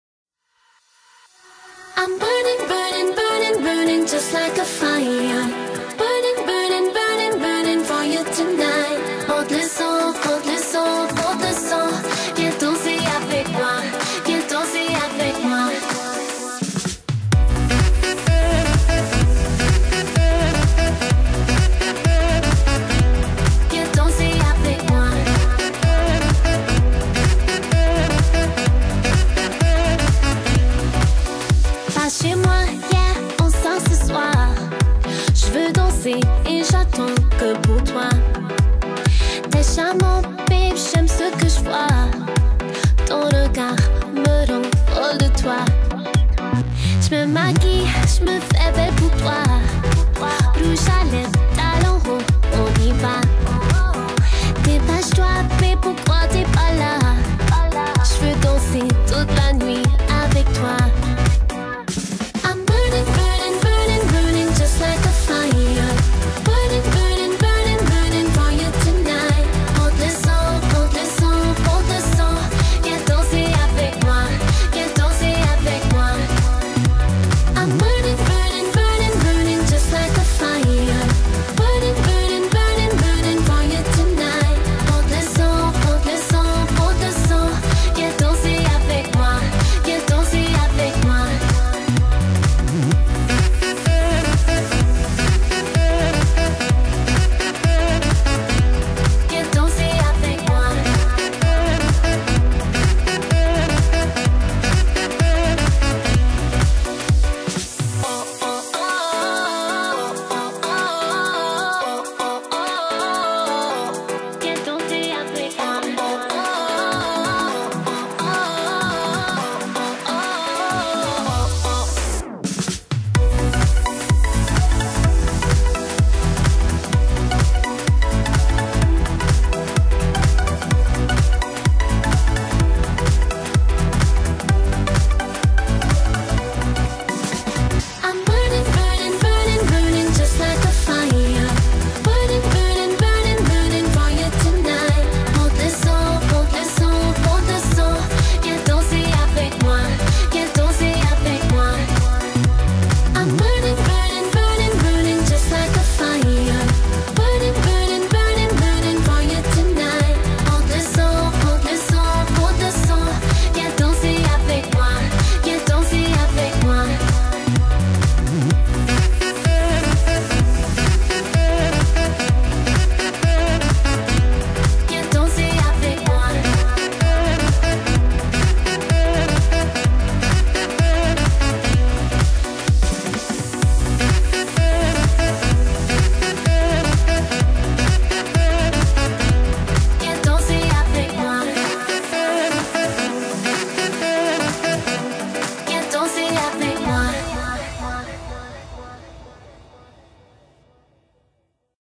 BURNING
Merci au danseurs: Claude, Louise, Nicole et André
BURNING LINE DANCE, 4 walls , 32 counts, beginner level LD, choreographed by Lyne Camerlain.